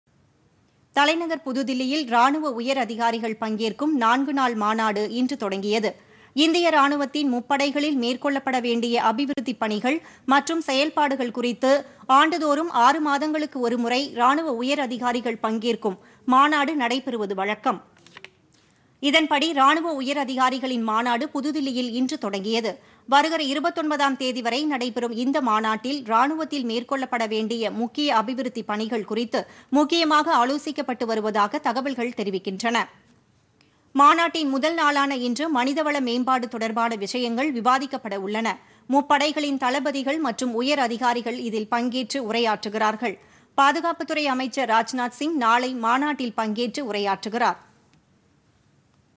ராணுவ உயர் அதிகாரிகள் 4 நாள் தேசிய மாநாடு
ராணுவ உயர் அதிகாரிகள் பங்கேற்கும் நான்கு நாள் தேசிய மாநாடு புதுதில்லியில் இன்று தொடங்கியது.